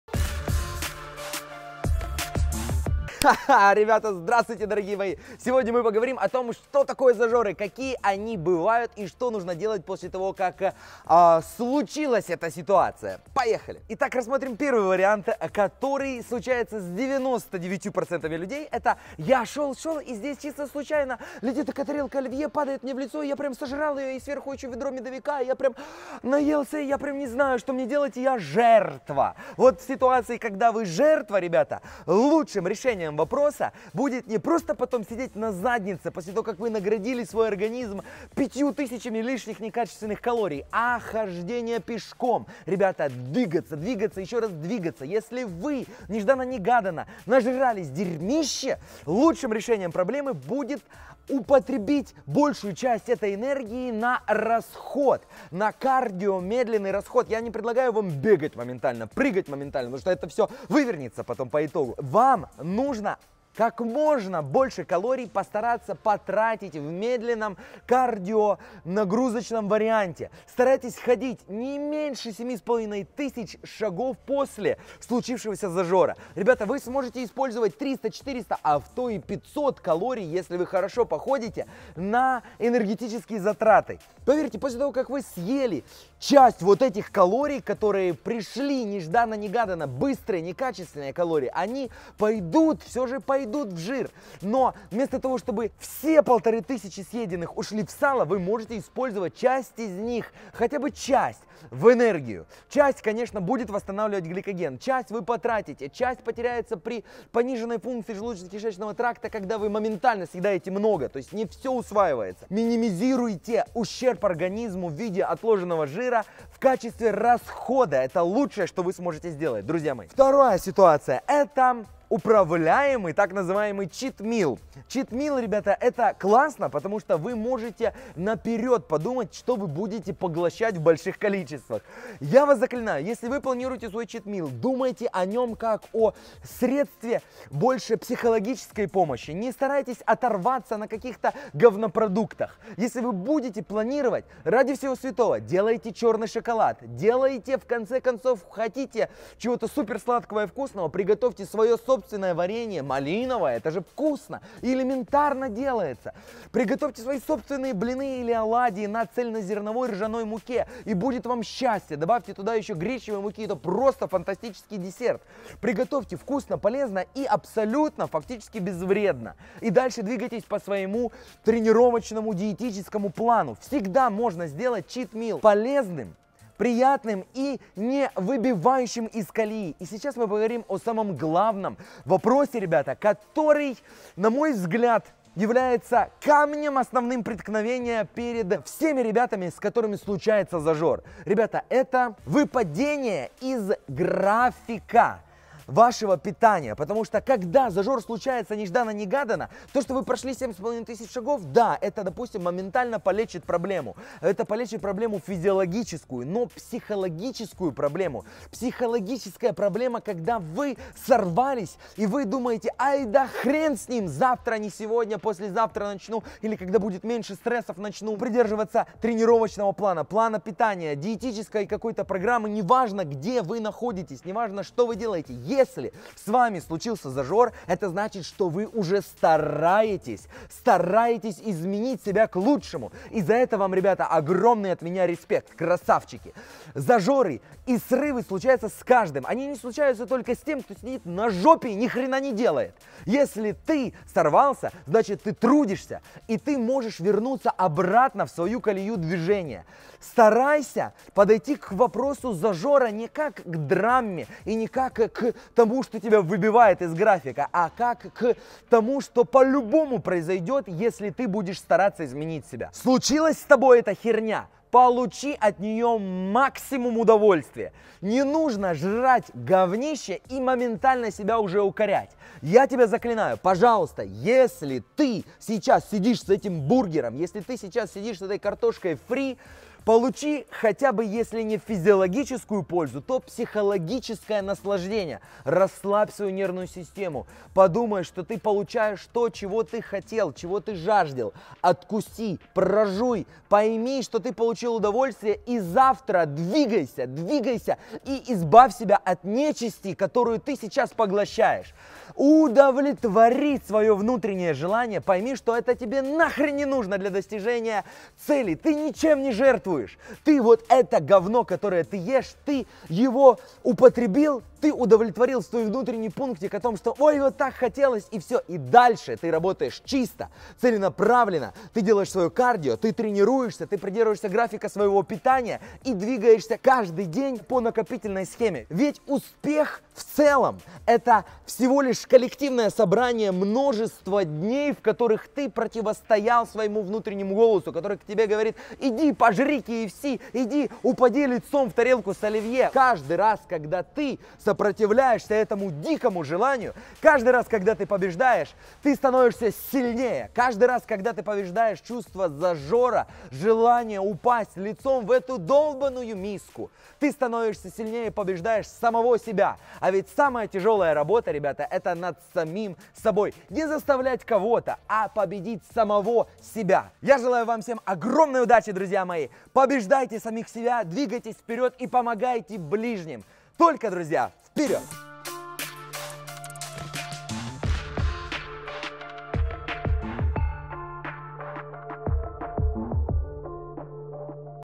Переедание и Зажоры | Срыв с диеты и как вернуться к здоровому питанию
Запись на персональную онлайн консультацию к эксперту по периодическому голоданию Алексею Ворону